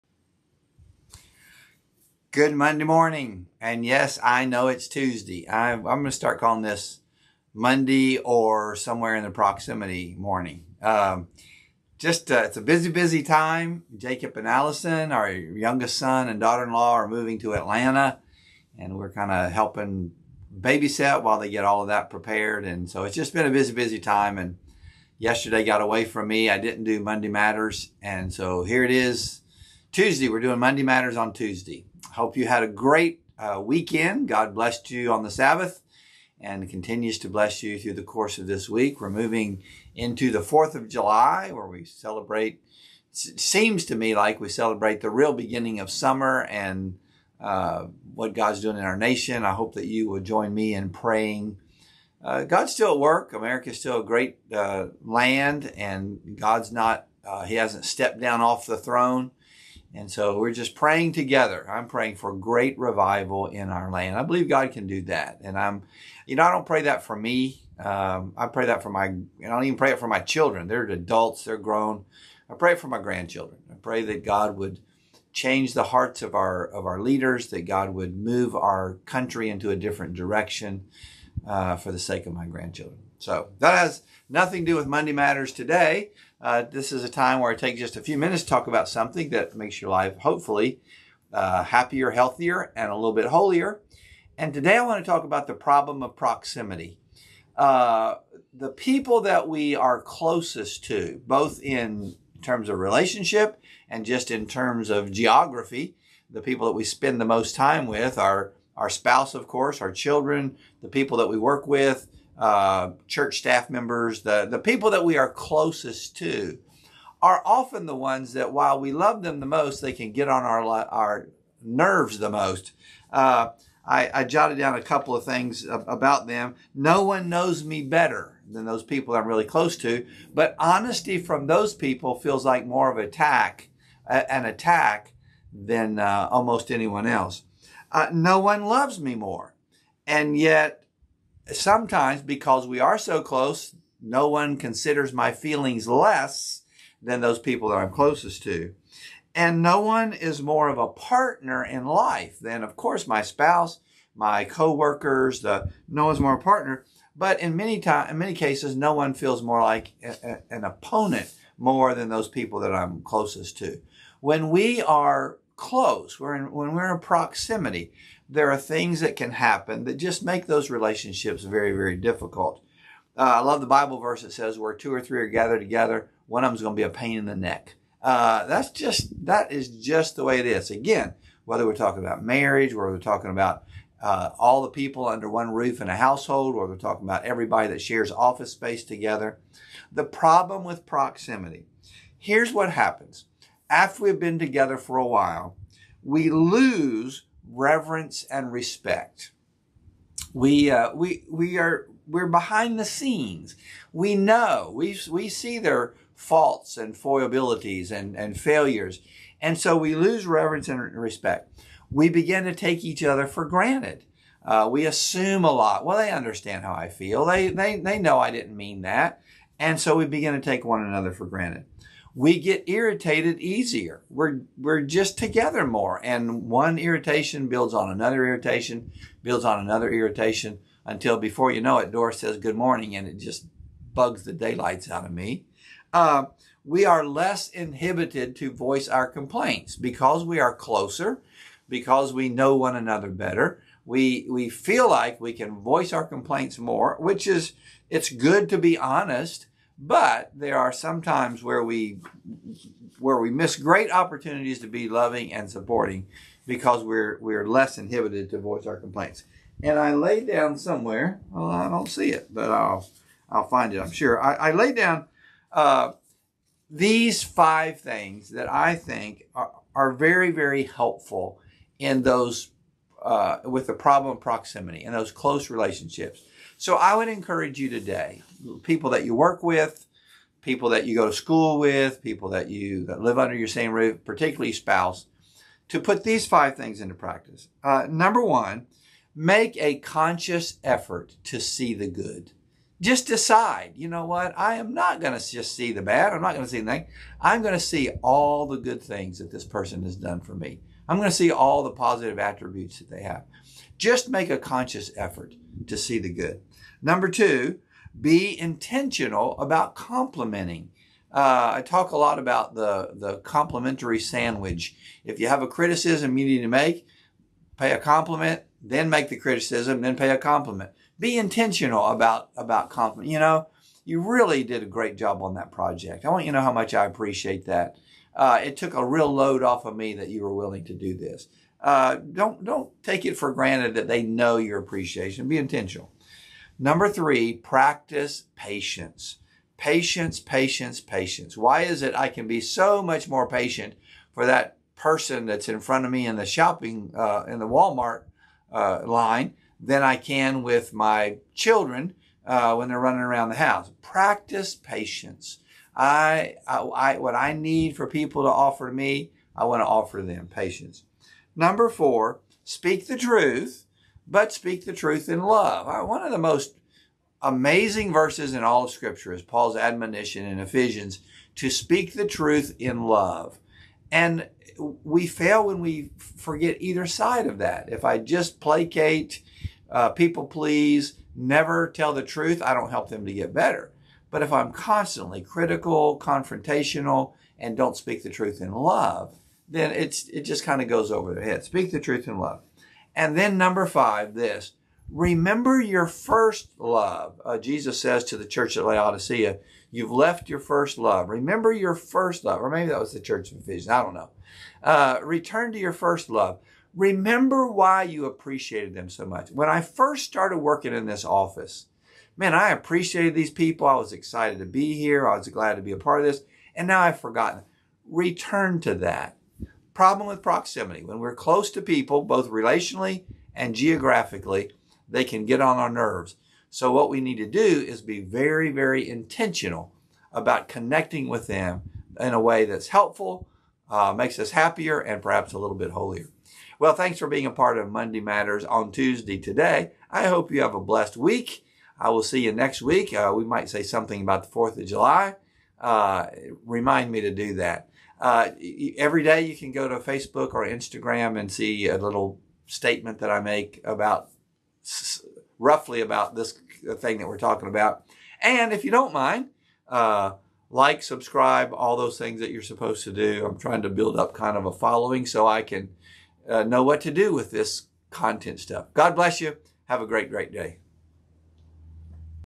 6/27/23 MondayMatters: The Problem with Proximity
Why is it that those who are closest to us can bug us the most? Why are the people we care about the hardest ones for us to be kind to? The more we are together, the more we see faults and flaws. And the more quickly we point them out. Here’s a gentle reminder to love the one you’re with. 😀